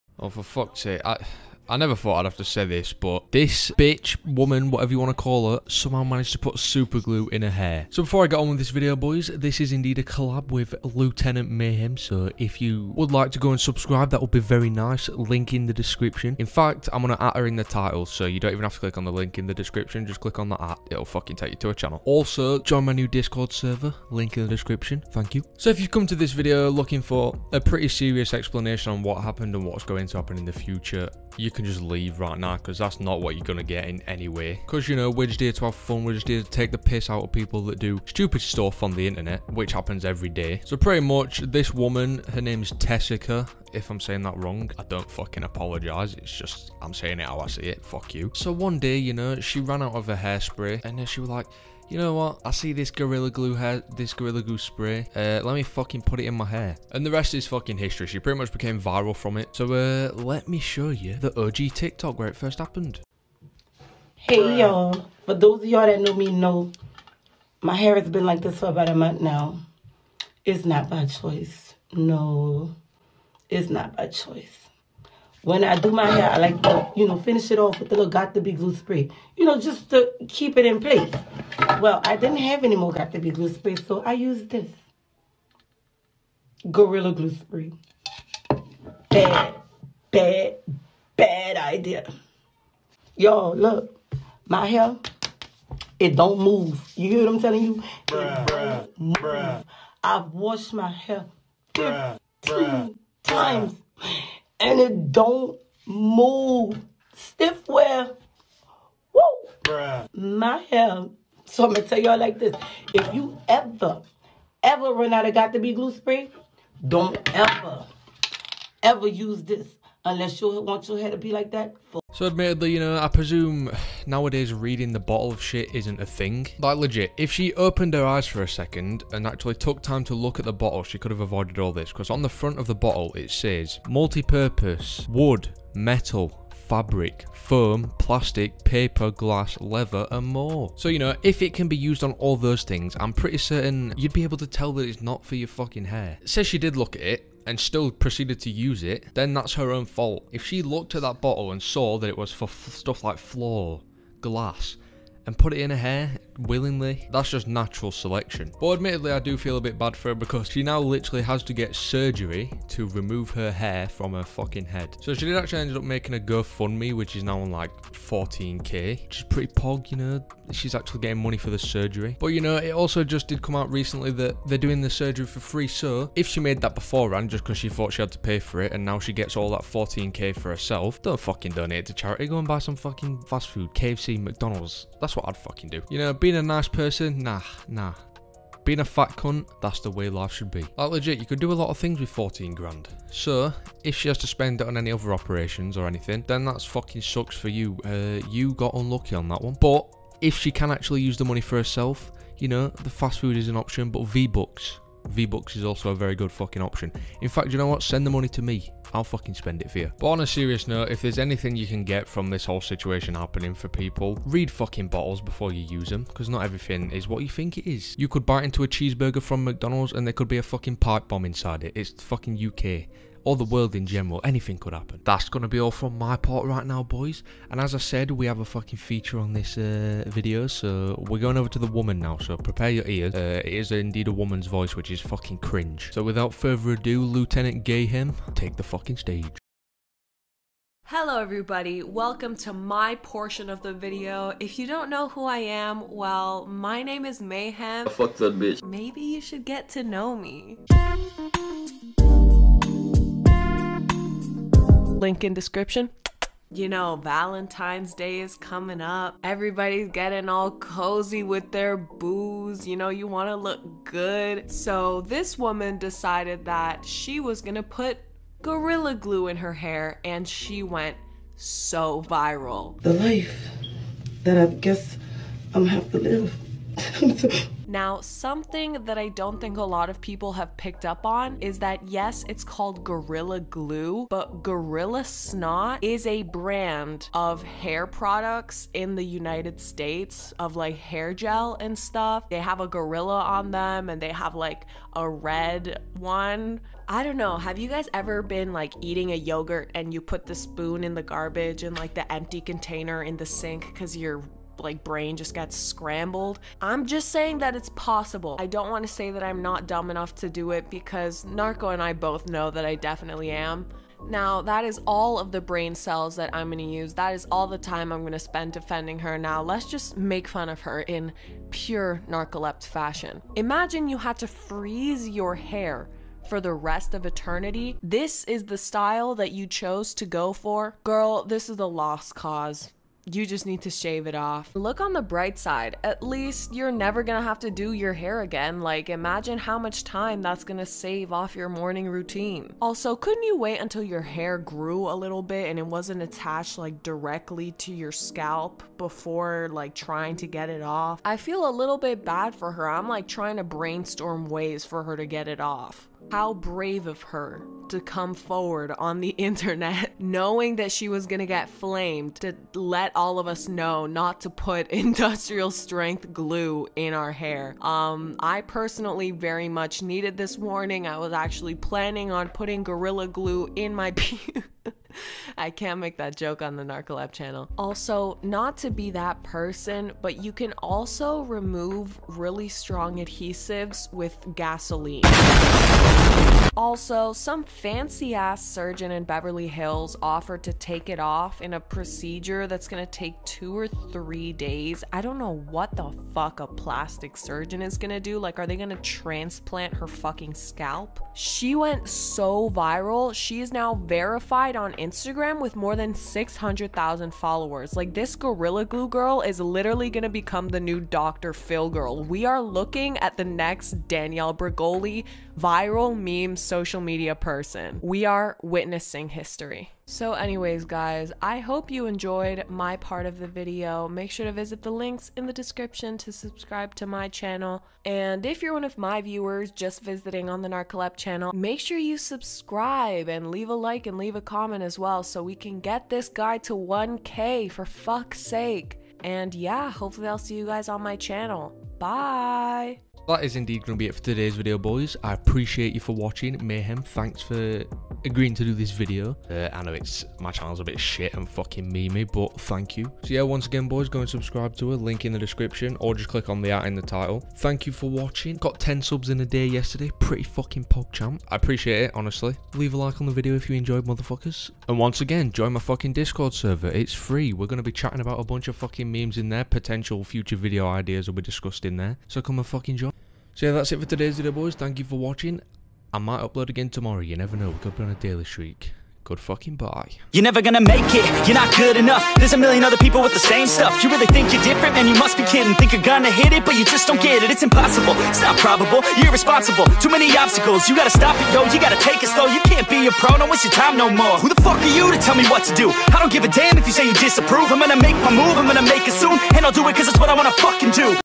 This Tiktoker Put Gorilla Glue In her Hair and Is THREATENING TO SUE (Tessica Brown AKA im_d_ollady)
gorilla glue girl instagram,
gorilla glue girl update,